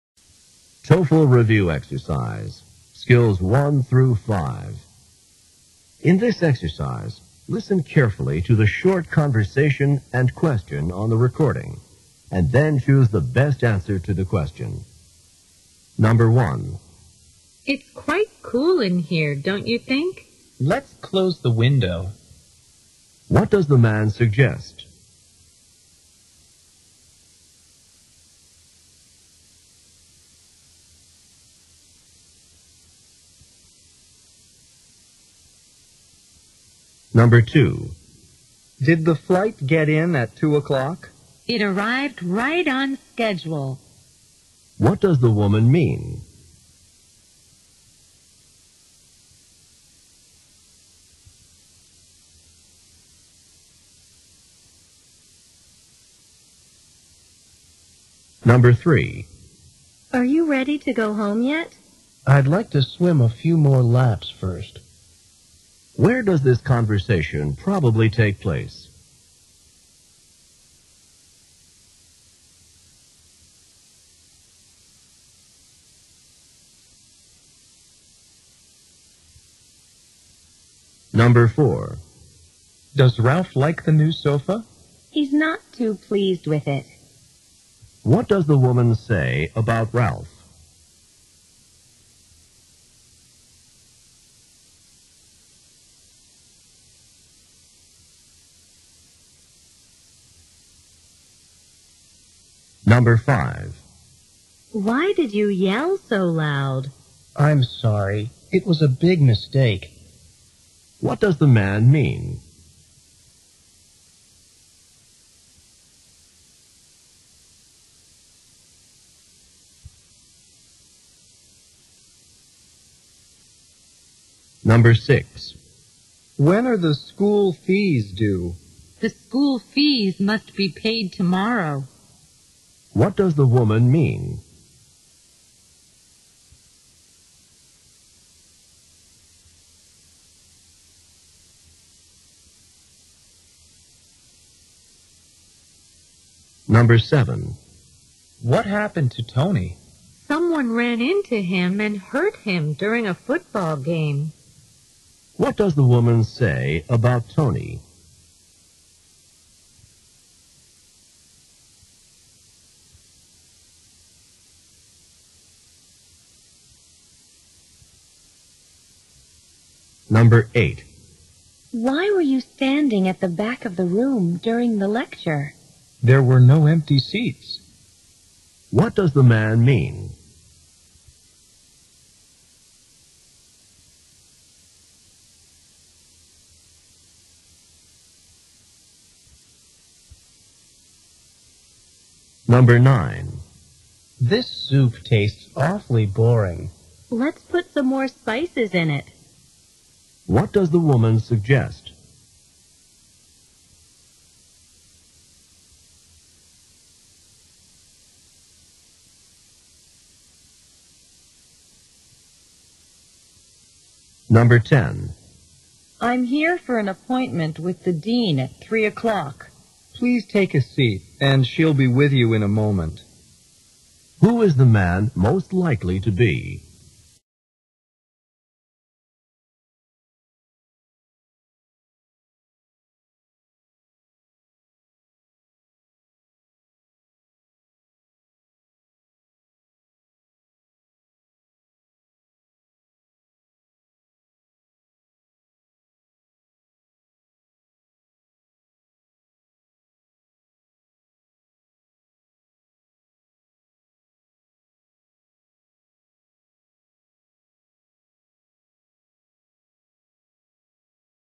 Toefl ITP / PBT Listening Short Conversations 4
More than 1.500 short conversations! Audio + Worksheets+Answers+transcripts.

This video contains 10 TOEFL short conversations for practice